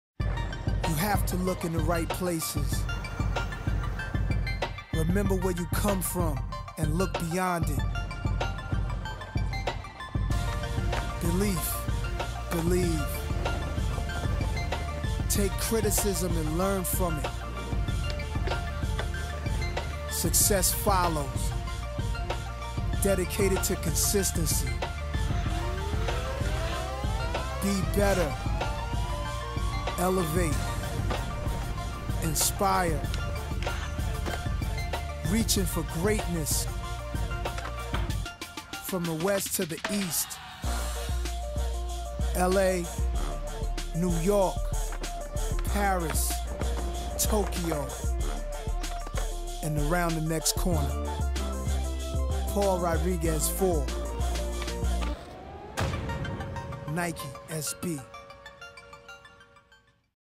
Nike SB Paul Rodriguez 4 x Nas Commerical
▷ About Paul:
Paul Rodriguez, also known as “P-Rod” was recognized as a prodigy at the young age of 14, only two years after he started skating.  His consistency, effortless style and persistence on his board led Paul to travel far from his home town of Northridge, CA to the City Streets around the world as a part of the legendary City Stars team.  His passion for skating and filming with his friends led to his breakout part in Logic 6 in 2000, Street Cinema in 2001 and landed him Rookie of the Year in 2002 for his part in Tranworld’s video production “In Bloom.” He turned pro later that year.

Paul bought his first skateboard at the age of 12.  When he’s not skateboarding, Paul enjoys studying the lives of influential individuals, including his favorites such as Alexander the Great and Bruce Lee.

Paul currently lives in Los Angeles.